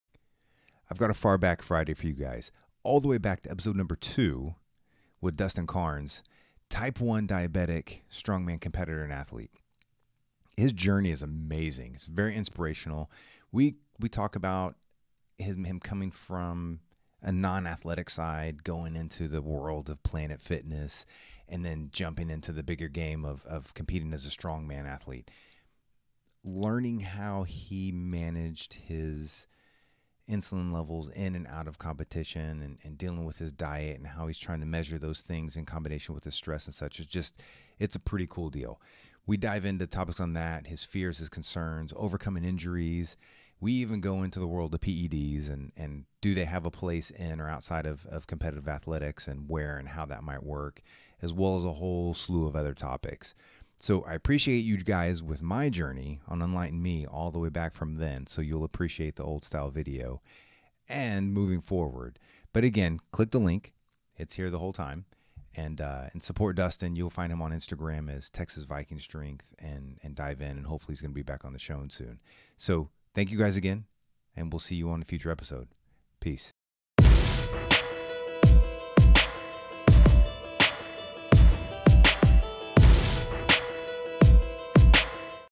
UME Shout out to Dustin Karnes - Diabetic Strongman! (Type 1)
UME 002 was a BLAST and I learned a ton about how he manages his insulin in and out of contests and training.  We dive into PED use in athletics, stories about injuries, diet and so much more!